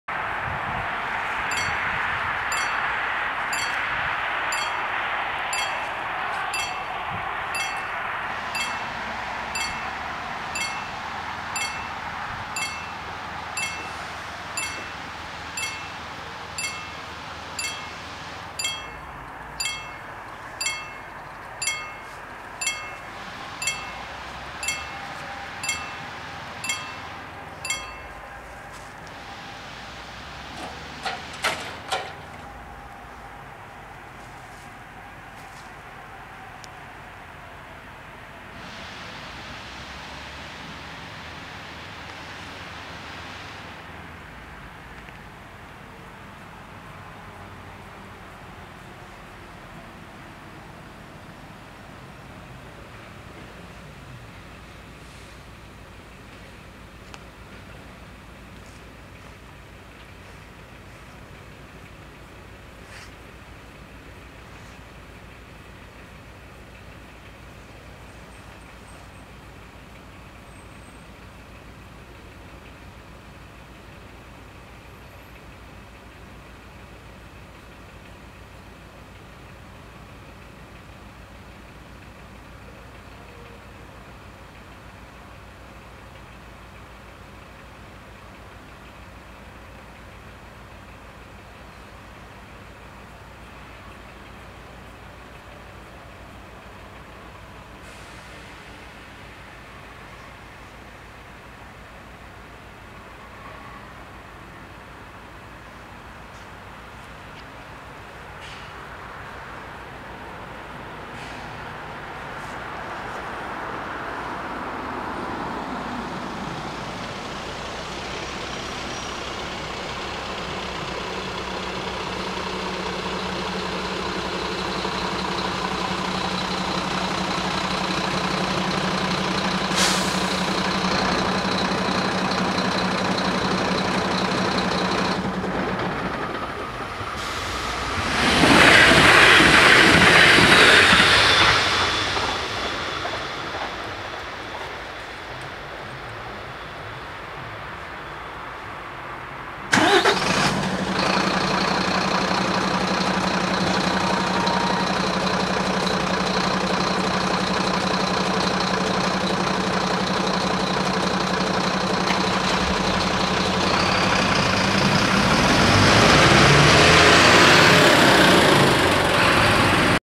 Spoorwegovergang Beutersitz (D) // Railroad crossing // Bahnübergang
Locatie: Berliner Straße, B101
Traject: Falkenberg - Cottbus
Soort: WSSB
Rode lichten: 2
Bellen: 2
Bomen: 2
Andreaskruisen: 4
Passeren:
- Talent2 als RE10 Cottbus → Leipzig